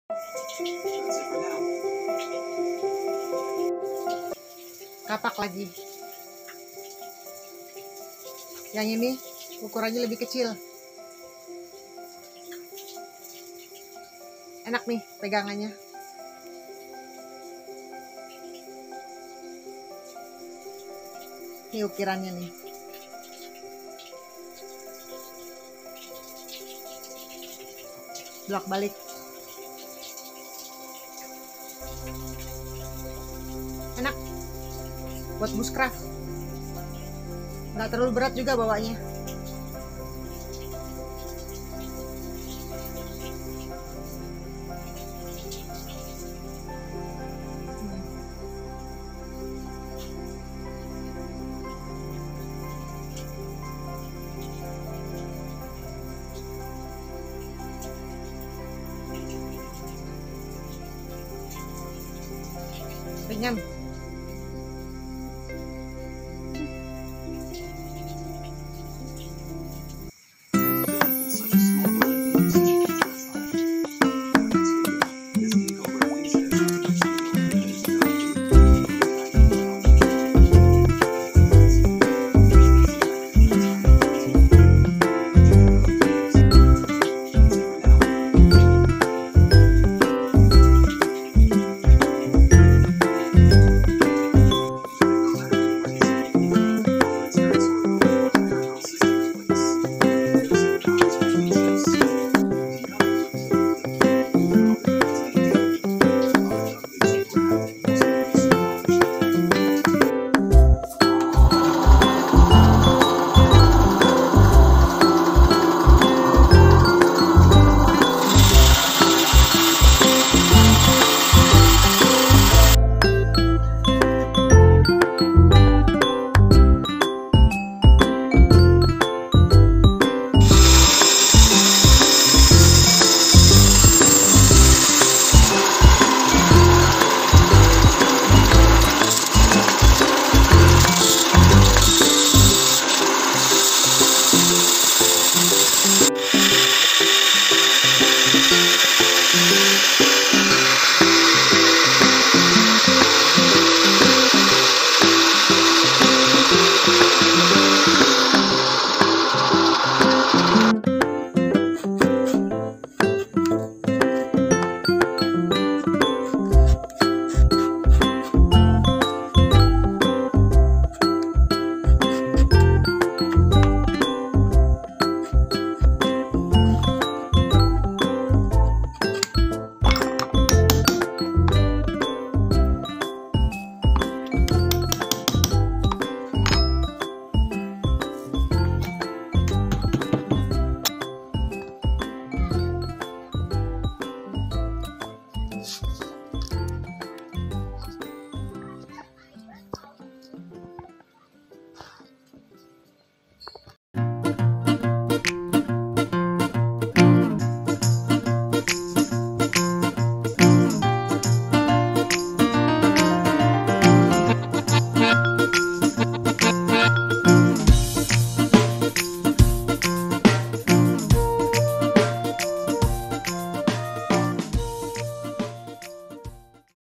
DSCA Collection | Kapak Bushcraft
DSCA Knives

Kapak bushcraft, dibuat dengan ukuran lebih kecil sehingga mudah dibawa untuk kegiatan outdoor atau bushcraft.
Pada gagang, kombinasi antara besi dengan kayu dan dilekatkan menggunakan Resin dan  3 buah pin dan Resin